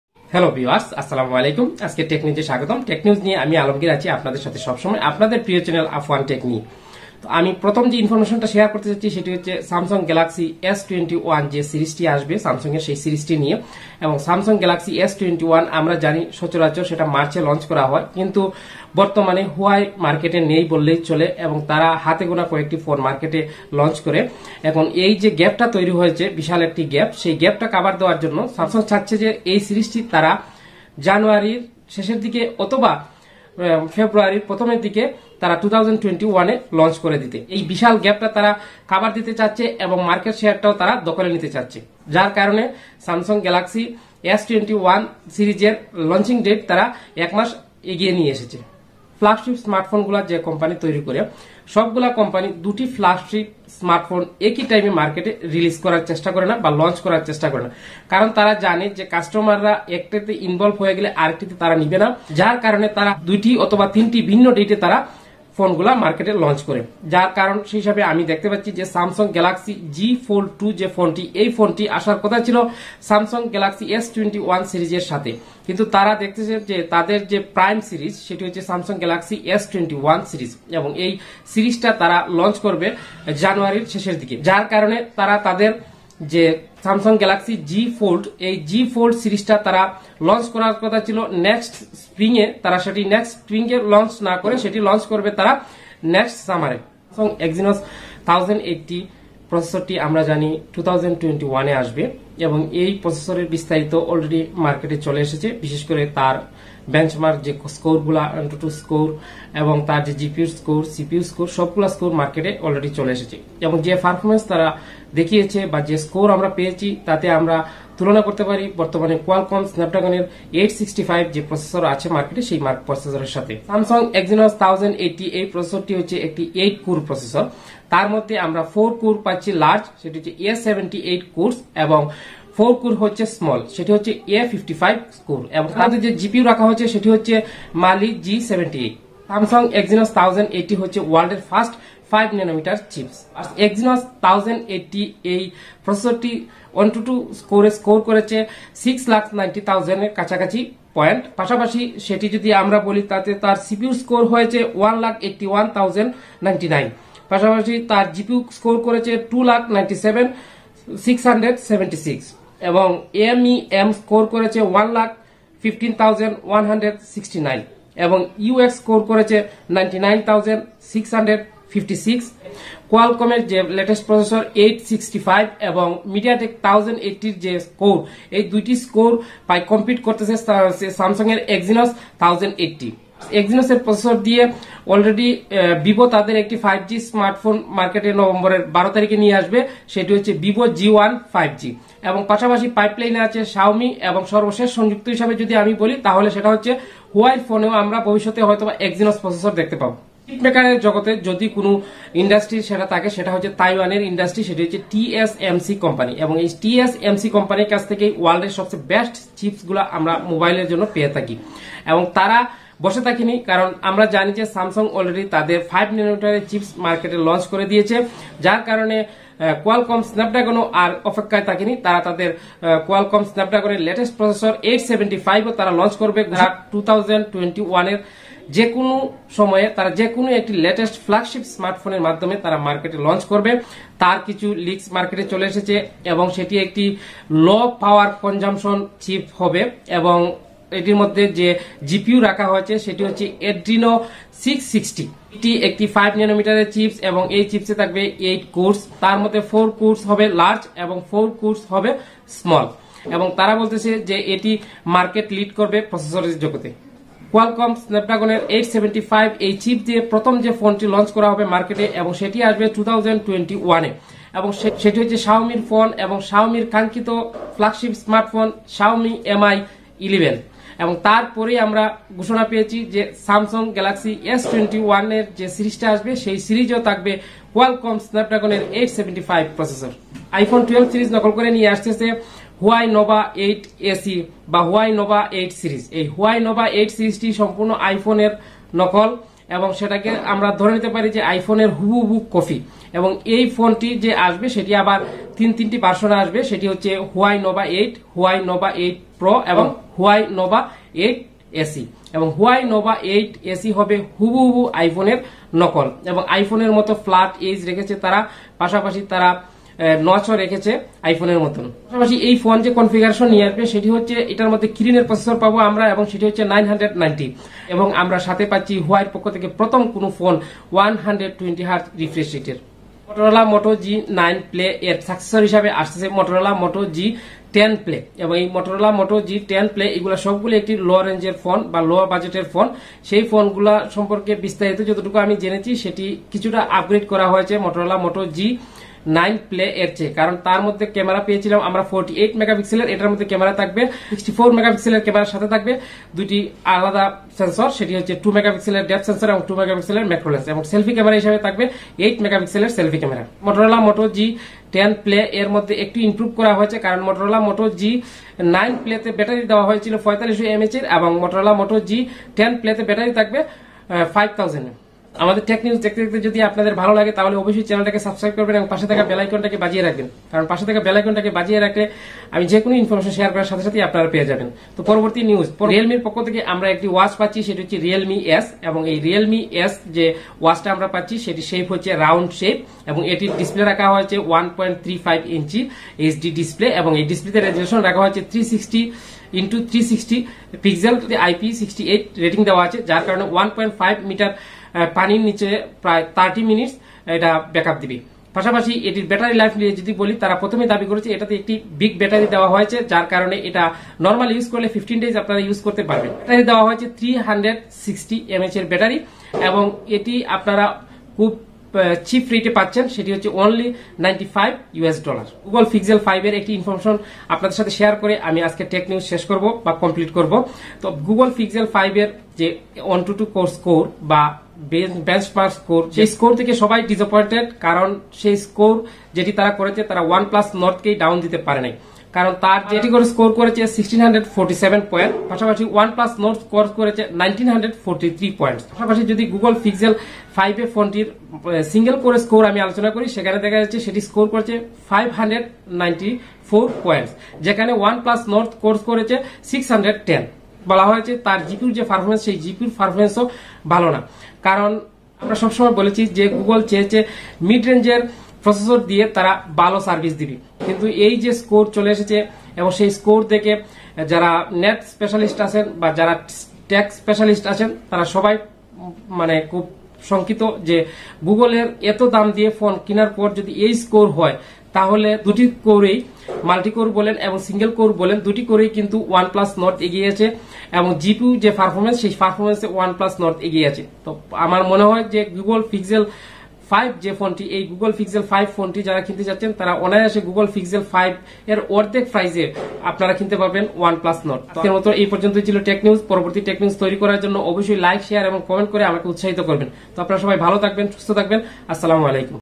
Google Pixel 5 কে হারিয়েছে OnePlus Nord। Iphone 12 সিরিজ কপি করেছে Huawei। Tech In Point
Hello Friends Welcome to Tech News No. 60 Today There Are Lots Of Amazing.